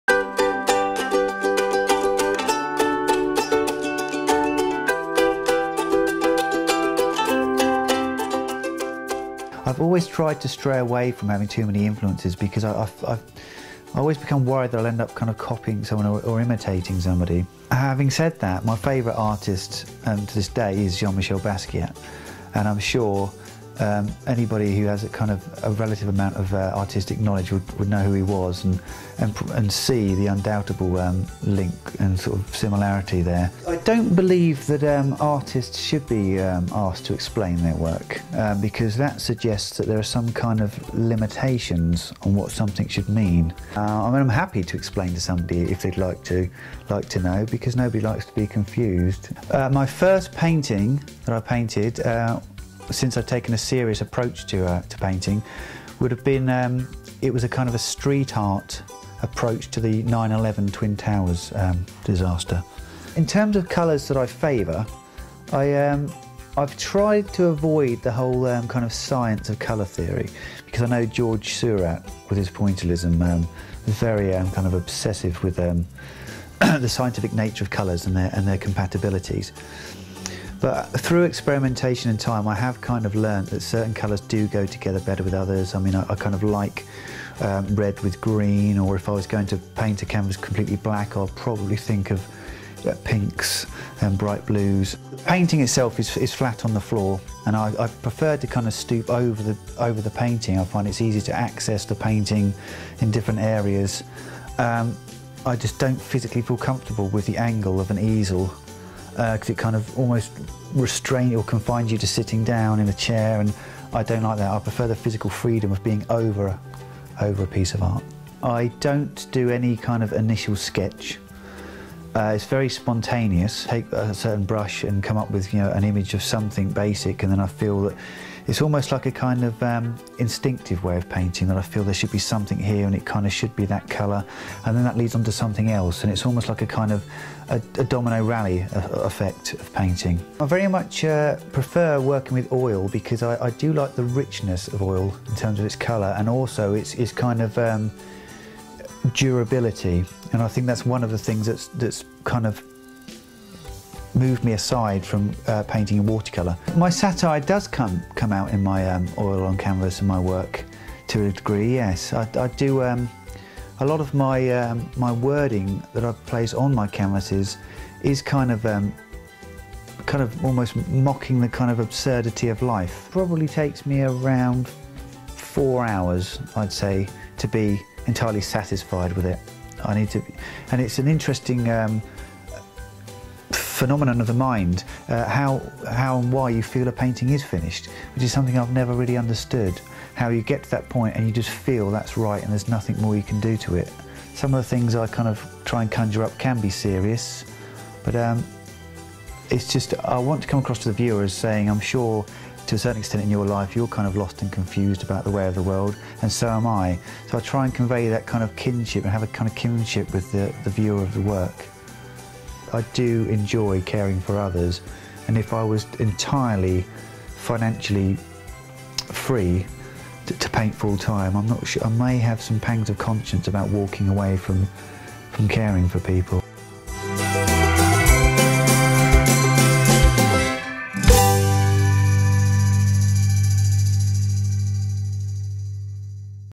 Charlie Chittenden, Part 2: The Art
Charlie Chittenden is a talented musician, song-writer and artist who has sold over 350 of his oil paintings throughout the world since 2009. He also works 50 hours a week in an EMI unit. In this film, he shares his enthusiasm for his art and talks about how he paints: his colours; styles; motivation and the process of producing an oil painting.